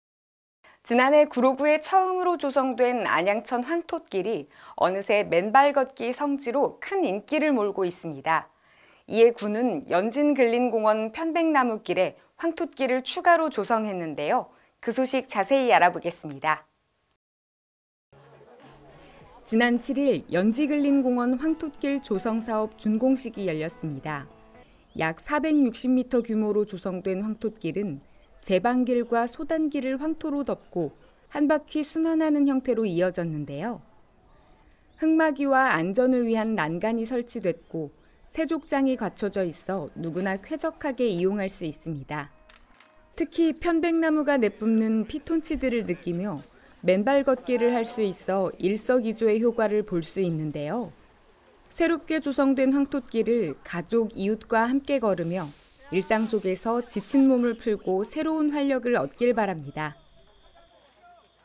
구로구, 연지근린공원에 황톳길 추가 조성
지난 7일, 연지근린공원 황톳길
조성사업 준공식이 열렸습니다.

약 460m 규모로 조성된 황톳길은
제방길과 소단길을 황토로 덮고
한 바퀴 순환하는 형태로 이어졌는데요,

흙막이와 안전을 위한 난간이 설치됐고,
세족장이 갖춰져 있어
누구나 쾌적하게 이용할 수 있습니다.

특히, 편백나무가 내뿜는
피톤치드를 느끼며 맨발 걷기를 할 수 있어
일석이조의 효과를 볼 수 있는데요,

새롭게 조성된 황톳길을 가족, 이웃과
함께 걸으며 일상 속에서 지친 몸을 풀고
새로운 활력을 얻길 바랍니다.